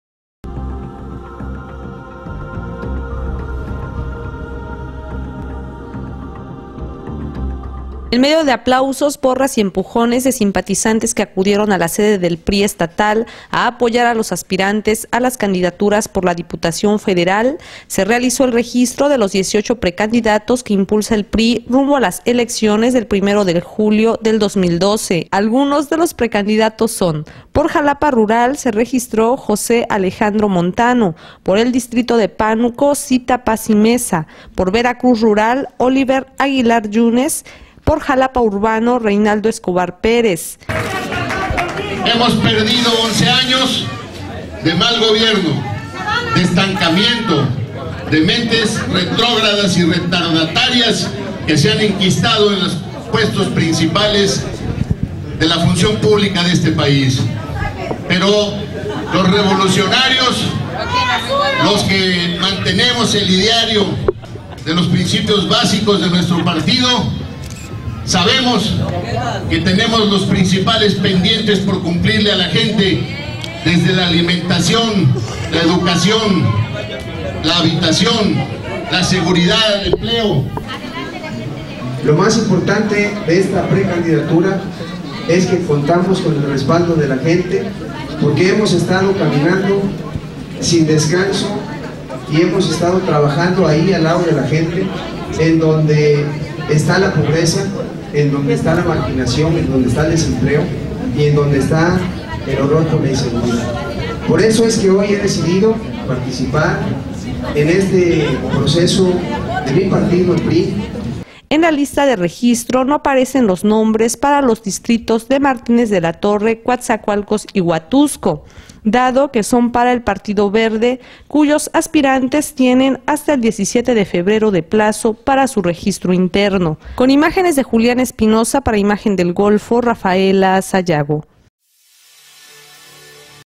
Se registran precandidatos diputación federal del PRI
En  medio de aplausos, porras y empujones de simpatizantes que acudieron a la sede del PRI estatal a apoyar a los aspirantes a las candidaturas por la diputación federal, se realizó el registro de los 18 precandidatos que impulsa el PRI rumbo a las elecciones del 1 de julio del 2012.
Algunos de los  precandidatos son: por  Xalapa Rural, se registró José Alejandro Montano, por el distrito de Pánuco, Zitta Pazzi Meza, por Veracruz Rural, Oliver Aguilar Yunes, por Xalapa Urbano Reynaldo Escobar Perez.